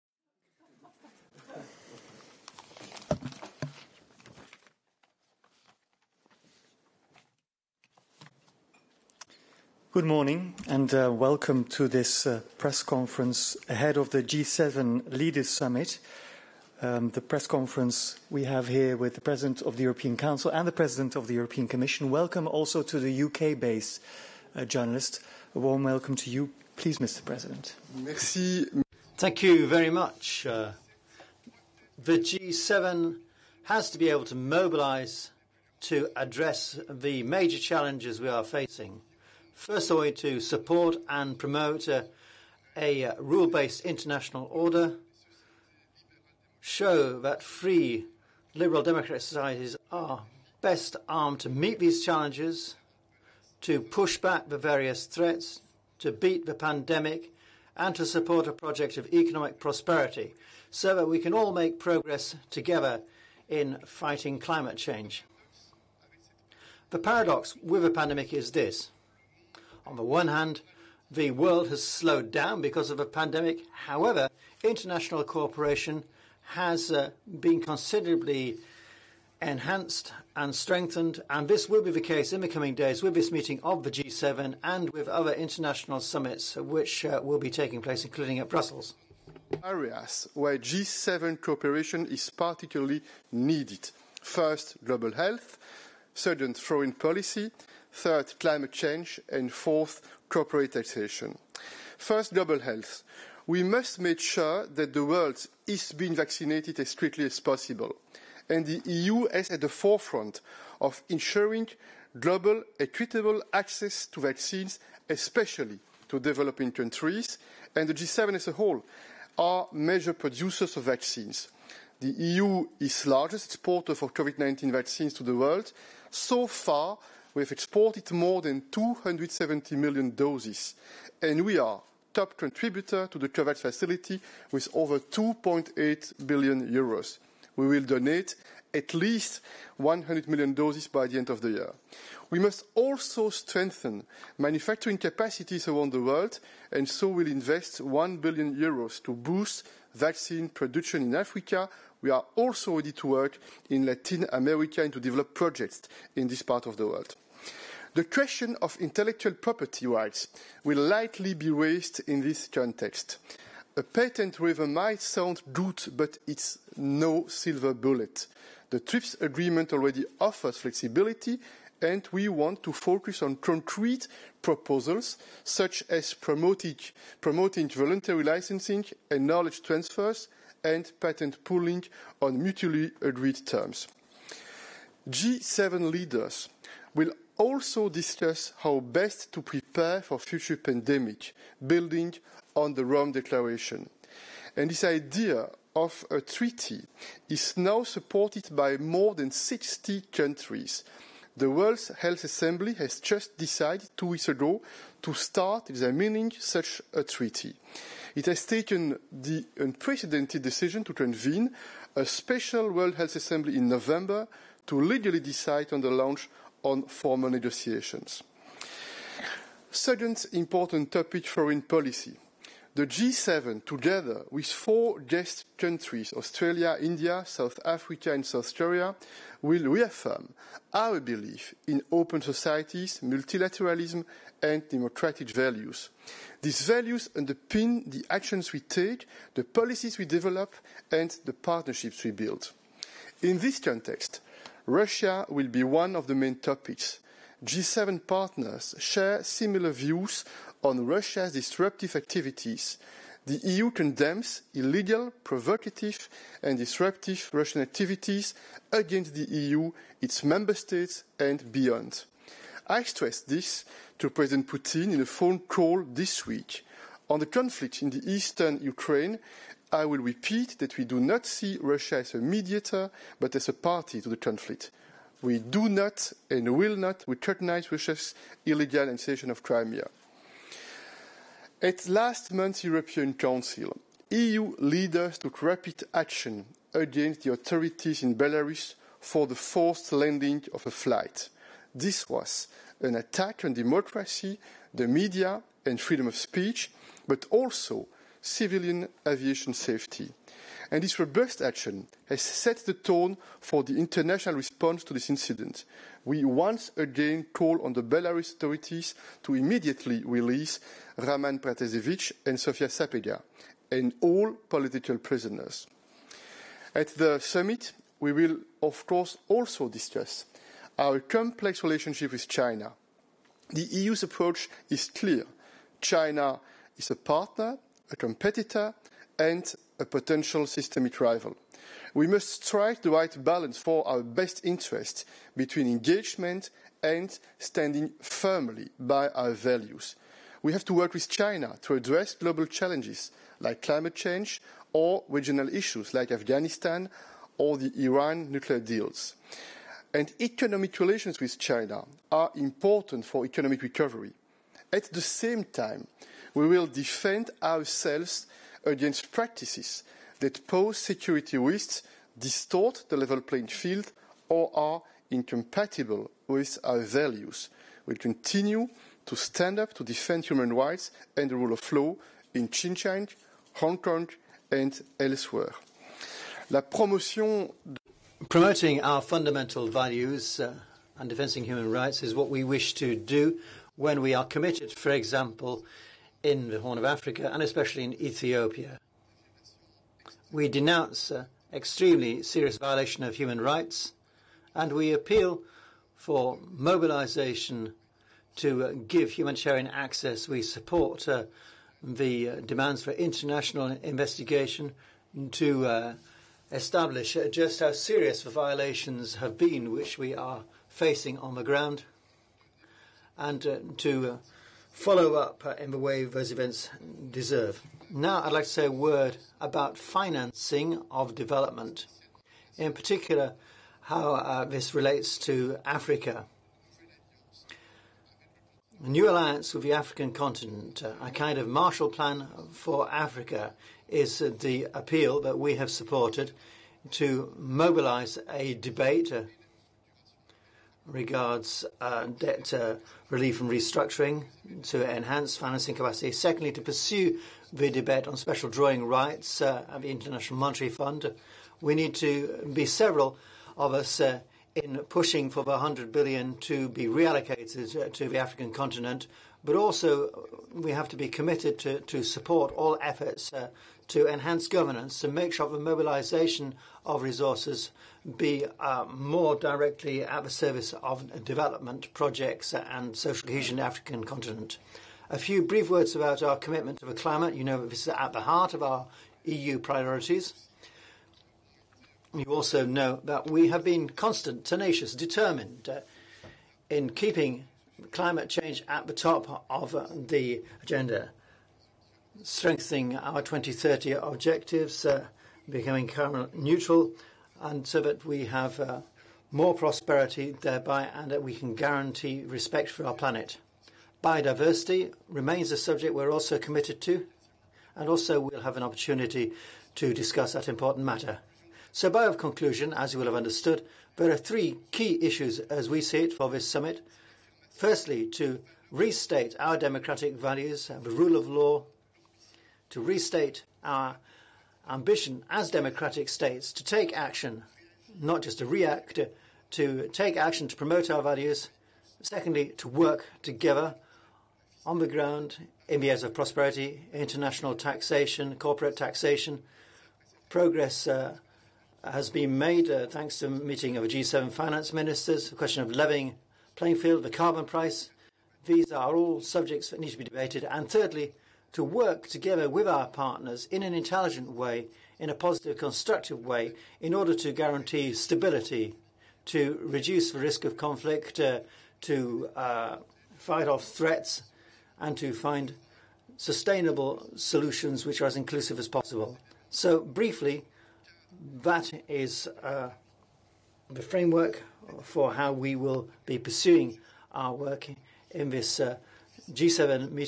President Charles Michel of the European Council debates ahead of the G7 summit
I would like to focus on four areas where G7 cooperation is particularly needed: first, global health; second, foreign policy; third, climate change; and fourth, corporatisation. First, global health. We must make sure that the world is being vaccinated as quickly as possible. The EU is at the forefront of ensuring global equitable access to vaccines, especially to developing countries. And the G7 as a whole are major producers of vaccines. The EU is the largest exporter of COVID-19 vaccines to the world. So far, we have exported more than 270 million doses and we are a top contributor to the Covax facility, with over 2.8 billion euros. We will donate at least 100 million doses by the end of the year. We must also strengthen manufacturing capacities around the world. So we will invest one billion euros to boost vaccine production in Africa. We are also ready to work in Latin America to develop projects in this part of the world.
The question of intellectual property rights will likely be raised in this context. A patent waiver might sound good, but it's no silver bullet. The TRIPS agreement already offers flexibility, and we want to focus on concrete proposals such as promoting voluntary licencing and knowledge transfers and patent pooling on mutually agreed terms. G7 leaders will also discuss how best to prepare for future pandemics, building on the Rome Declaration. And this idea of a treaty is now supported by more than 60 countries. The World Health Assembly has just decided two weeks ago to start examining such a treaty. It has taken the unprecedented decision to convene a special World Health Assembly in November to legally decide on the launch of formal negotiations.

Second important topic, foreign policy. The G7, together with four guest countries, Australia, India, South Africa and South Korea, will reaffirm our belief in open societies, multilateralism and democratic values. These values underpin the actions we take, the policies we develop, and the partnerships we build. In this context, Russia will be one of the main topics. G7 partners share similar views on Russia's disruptive activities. The EU condemns illegal, provocative and disruptive Russian activities against the EU, its member states and beyond. I stressed this to President Putin in a phone call this week. On the conflict in eastern Ukraine, I will repeat that we do not see Russia as a mediator, but as a party to the conflict. We do not and will not recognise Russia's illegal annexation of Crimea.

At last month's European Council, EU leaders took rapid action against the authorities in Belarus for the forced landing of a flight. This was an attack on democracy, the media and freedom of speech, but also civilian aviation safety. And this robust action has set the tone for the international response to this incident. We once again call on the Belarus authorities to immediately release Roman Protasevich and Sofia Sapega and all political prisoners. At the summit we will, of course, also discuss our complex relationship with China. The EU's approach is clear: China is a partner, a competitor and a potential systemic rival. We must strike the right balance for our best interests between engagement and standing firmly by our values. We have to work with China to address global challenges, like climate change, or regional issues, like Afghanistan or the Iran nuclear deal. And economic relations with China are important for economic recovery. At the same time, we will defend ourselves against practises that pose security risks, distort the level playing field, or are incompatible with our values. We will continue to stand up to defend human rights and the rule of law in Xinjiang, Hong Kong and elsewhere.

La promotion de nos valeurs fondamentales, la défense des droits humains, c'est ce que nous voulons faire aussi quand nous sommes engagés, par exemple, dans la Corne de l'Afrique et spécialement en Éthiopie. Nous dénonçons des violations extrêmement graves des droits humains et nous faisons appel à la mobilisation pour donner les accès humanitaires. Nous soutenons la demande pour des enquêtes internationales afin de qualifier les éléments graves de violations auxquels nous sommes confrontés sur le terrain, et de donner les conséquences que méritent ces évènements.